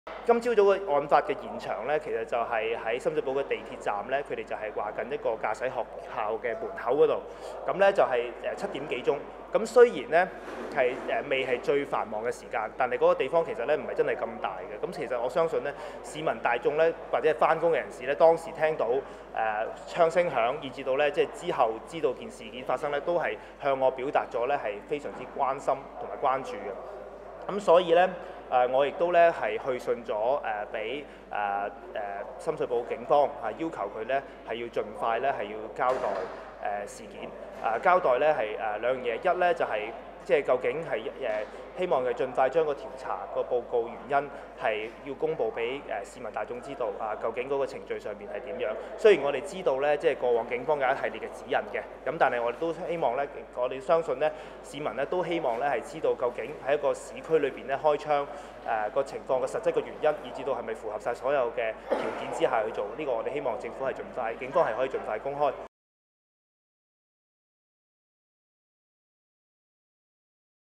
【警港鐵站開槍】鄭泳舜:要求交代鬧市開槍是否符指引
20181108

深水埗港鐵站昨晨上班時間發生機動部隊女警開槍阻截鎅刀漢事件。機動部隊（PTU）警員早上在站內截查一名55歲男子時，男子在胸前背包拿出一把15厘米長鎅刀衝向警員，勸喻不果後女警擎槍射一槍，擊中男子腹部。今次是首度有警員在港鐵站內開槍，也是女警自1990年代中可佩槍以來，首度有女警執勤時開槍制服疑人。